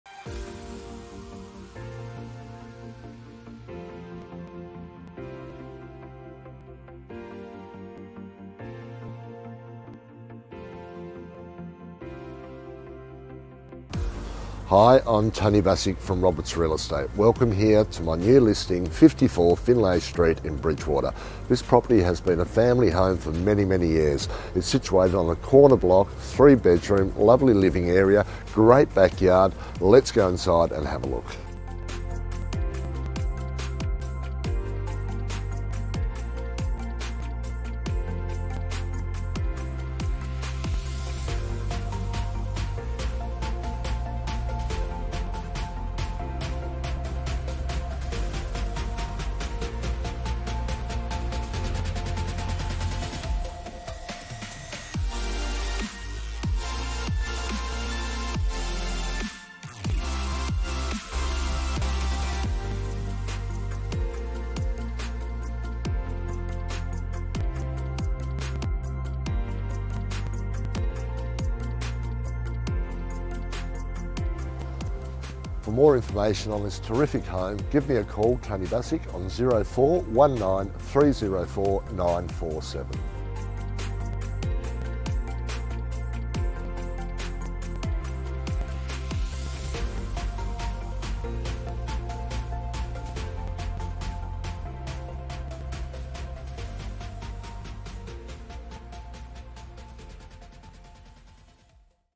54 Finlay Street Bridgewater
This 3-bedroom home has been in the same family for many years, situated on a corner block with a fenced yard, undercover parking, and outside sheds.
The home has just been repainted, with new carpets and flooring throughout, as well as new fittings.  The living area is separate from the kitchen and dining combined, a great home where you can just walk in and enjoy.
The home will suit numerous buyers from first home buyers to retirees or investors.  The rental potential is $420-$430 per week.